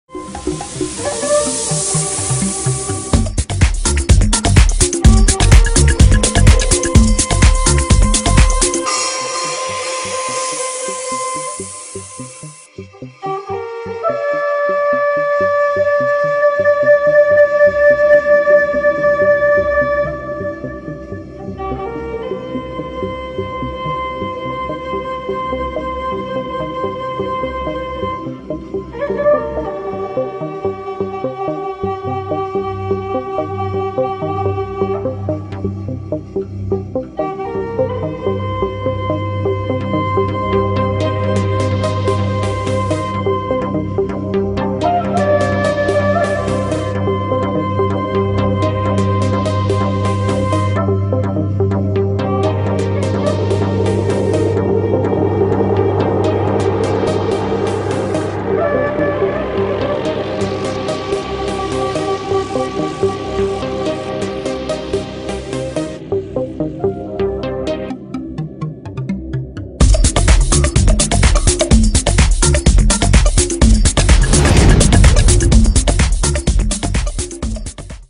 GoPro Hero - Timelapse - (Bogota)
A timelapse sequence testing the GoPro Hero 2
Location: Bogota, Colombia
Equipment: GoPro Hero 2

Some places in the timelapse:
- On the road from Bogota to Zipaquira
- Monserrate
- Los Heroes Monument
- 15 Av with 116 st intersection
By:
Carlos Bolivar